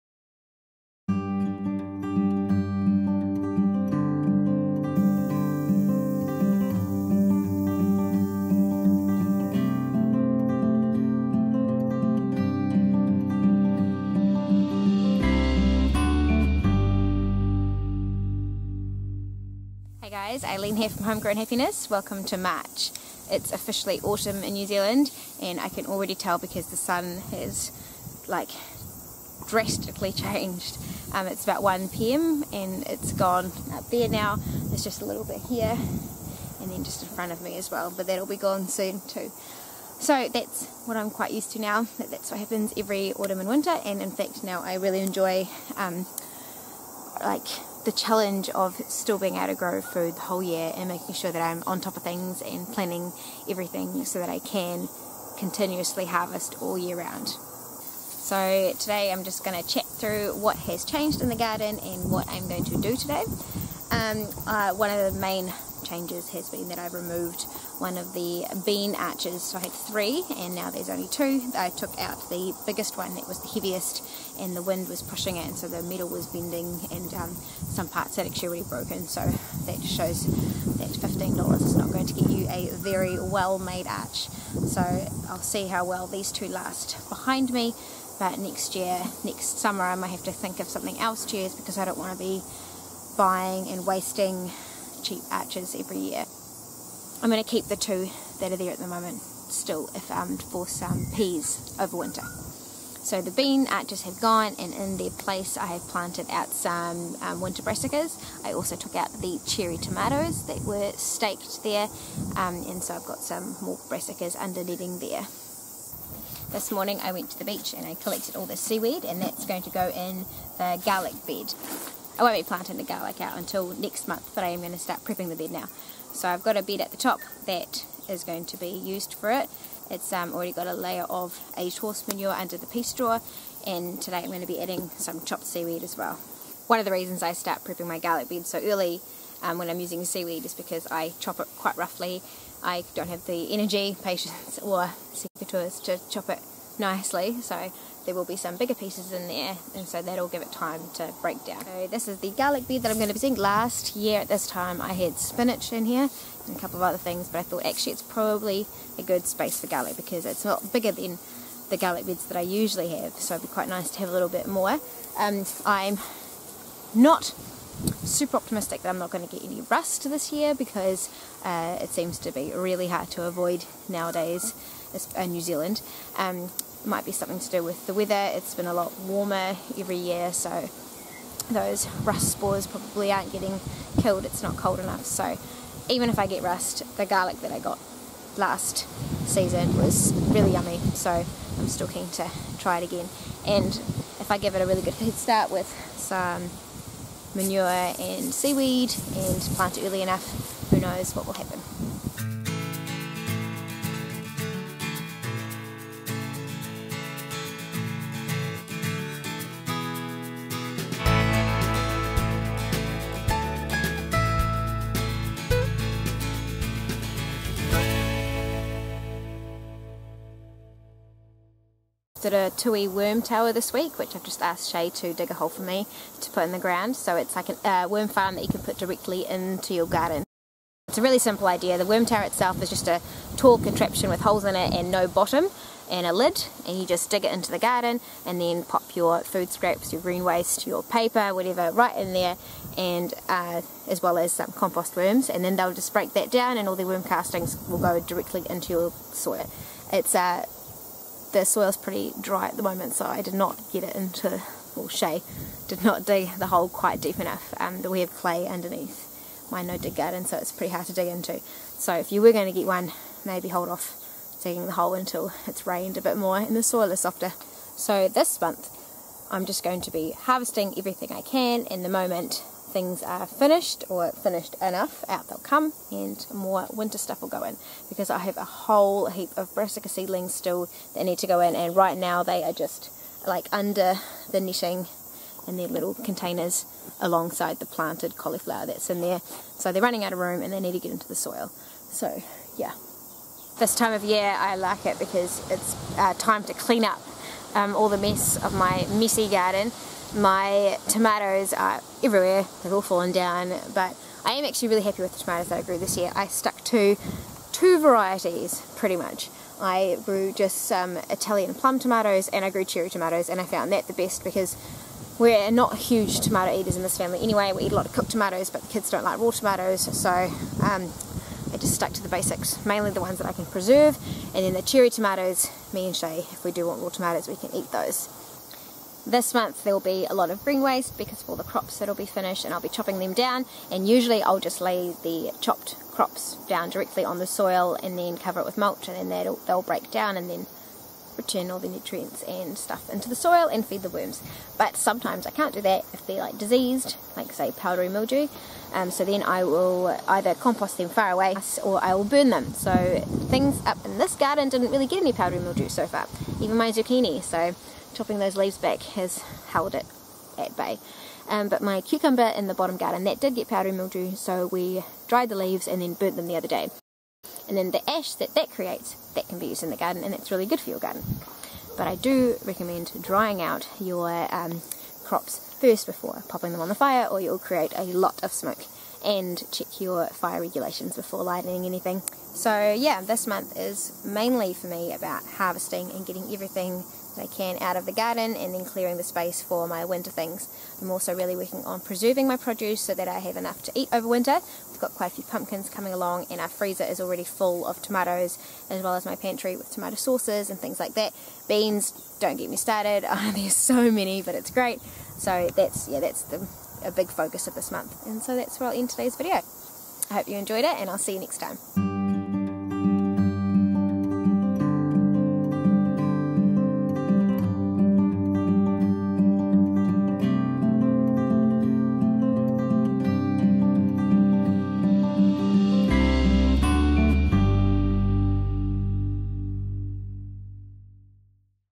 My No-Dig Garden // Autumn // March 2019 // New Zealand
This month I need to harvest as much as I can and clear space for winter crops! I've got some seaweed for the garlic bed and I'll show what I do with my diseased crop.